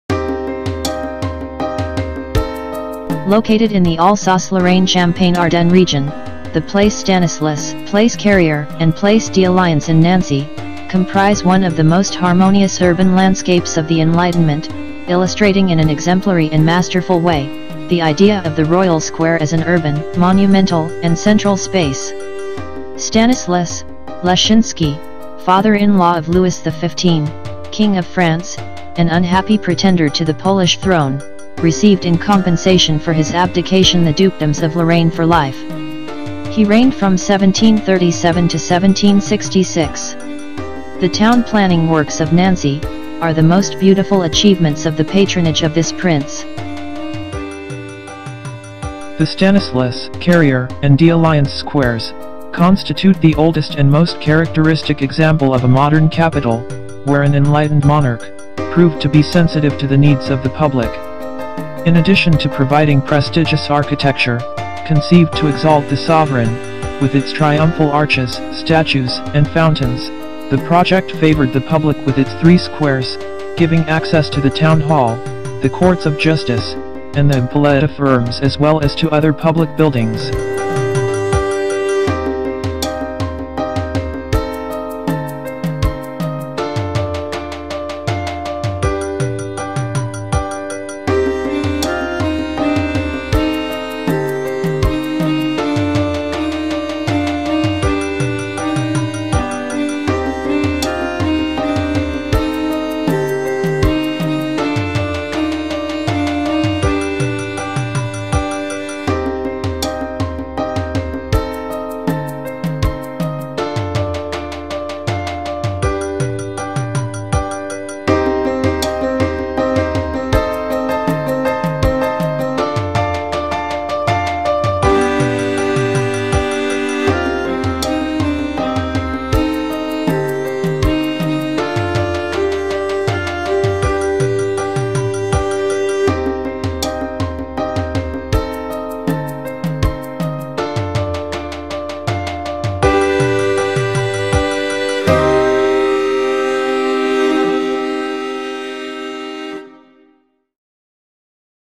France - Place Stanislas, Place de la Carrière, Place d'Alliance in Nancy
Place Stanislas, Place de la Carrière, Place d'Alliance in Nancy - France